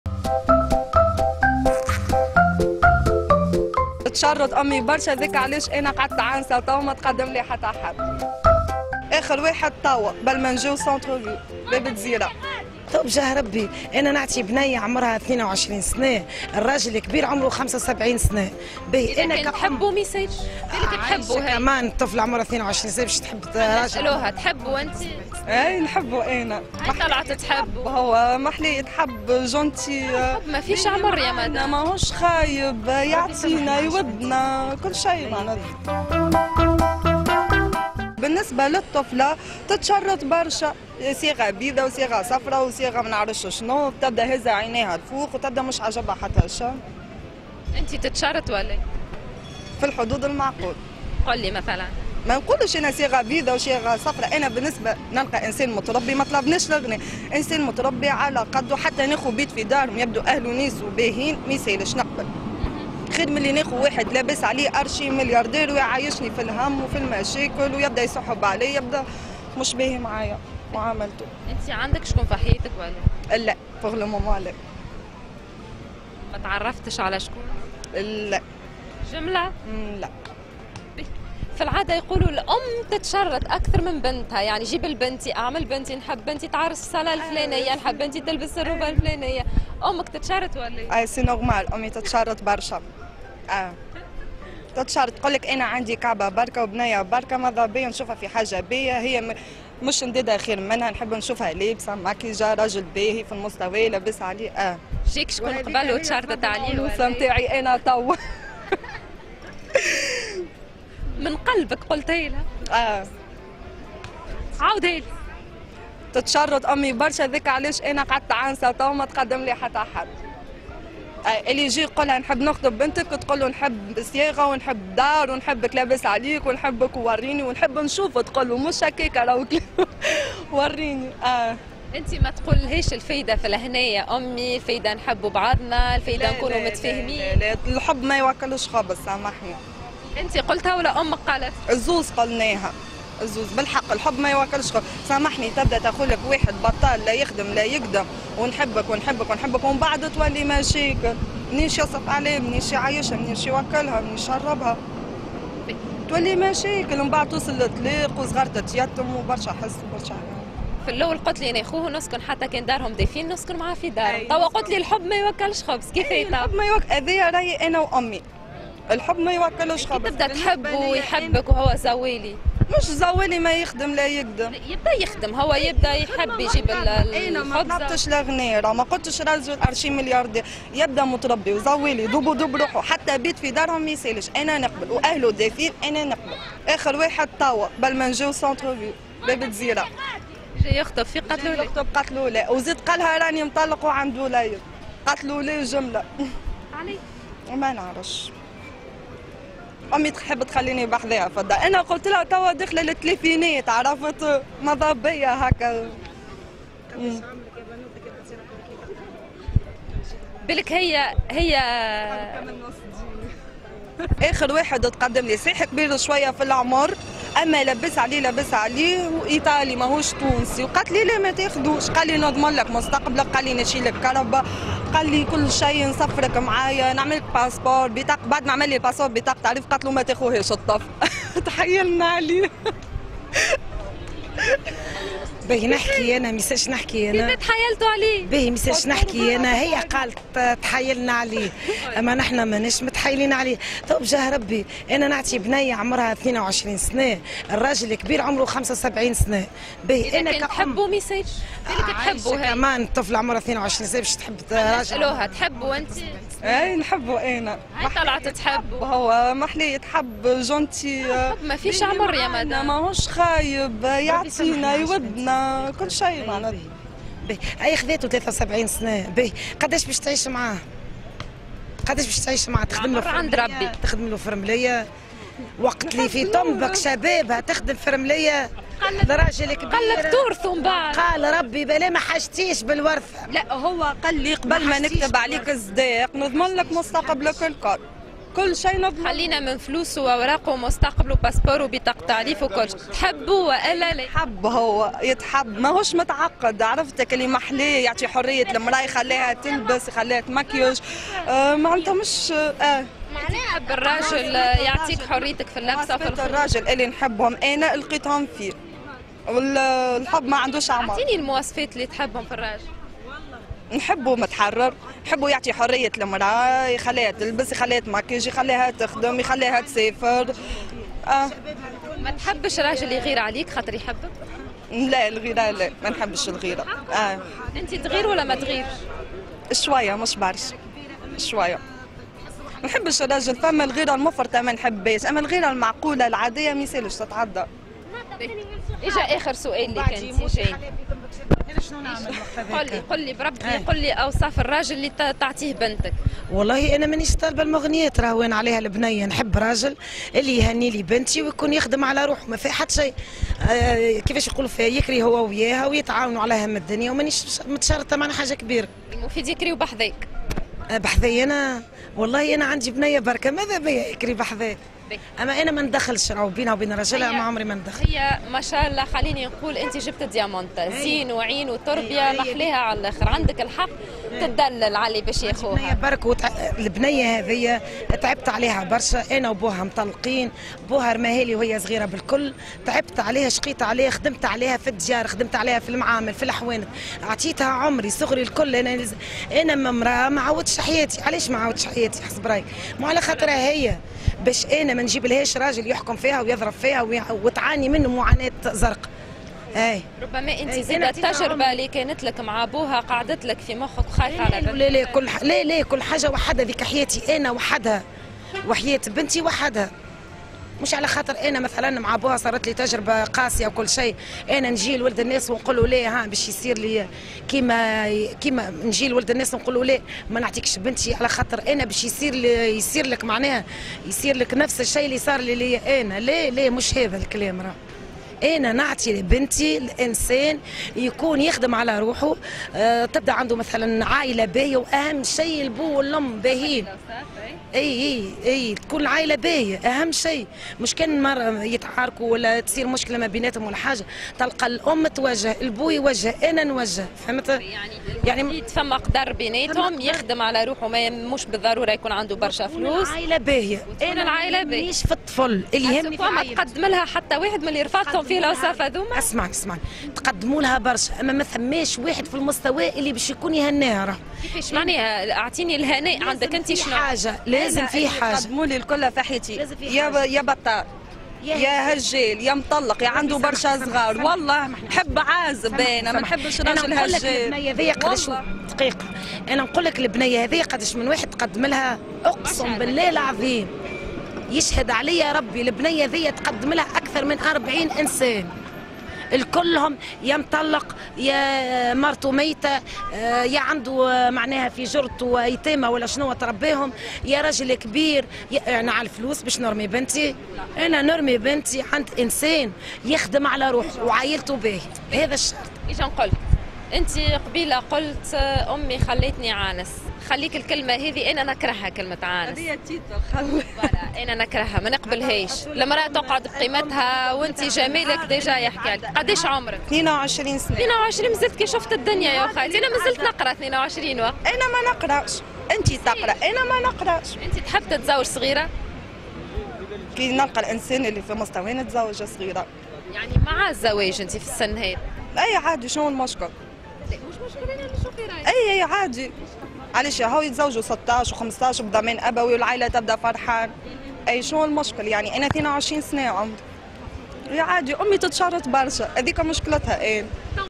Réalitétunisienne😂 أمي تشرط برشا خلتني عانس أنا نحب واحد عمرو 75 سنة
الشارع التونسي العنوسة في تونس 2021